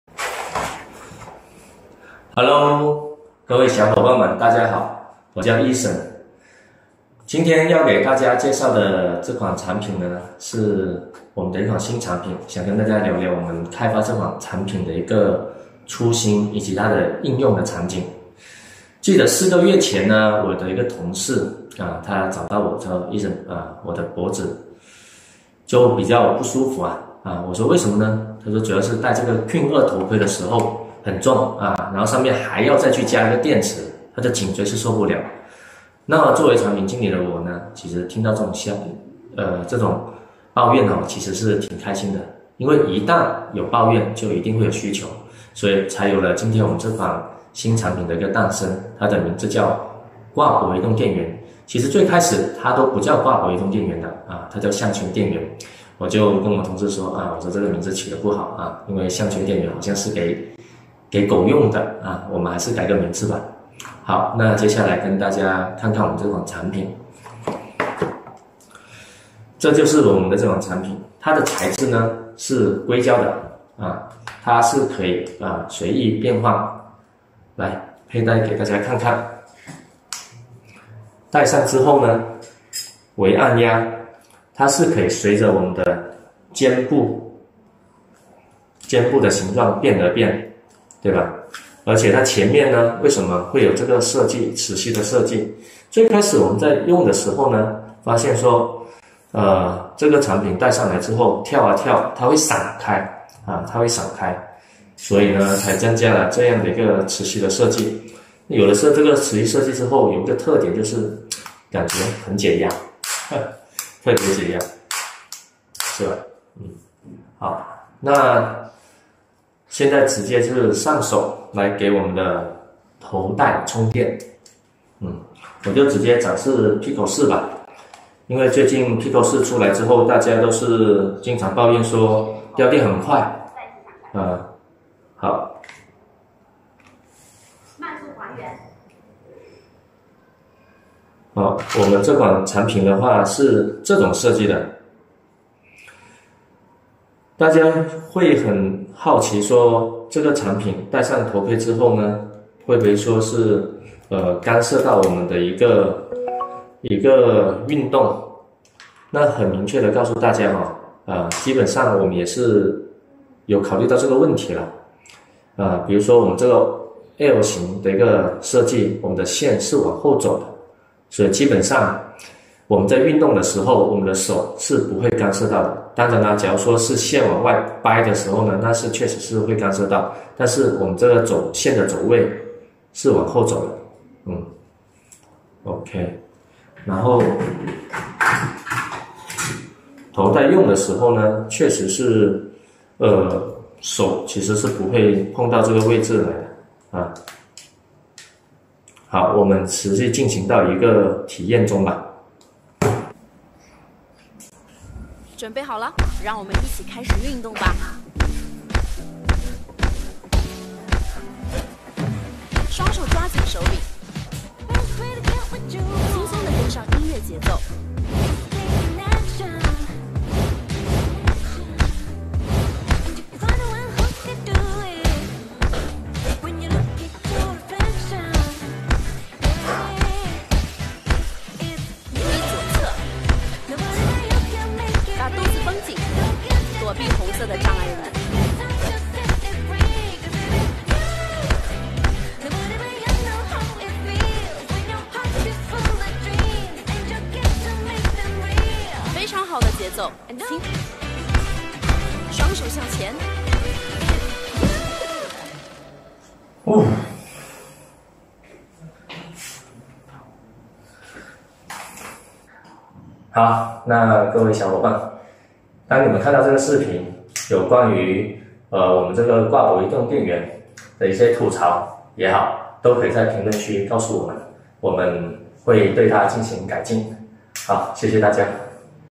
AMVR Quest 2 挂脖移动电源 Neck Handing Powerbank for Quest 2
谈谈关于开发这款产品的初心，以及应用的场景。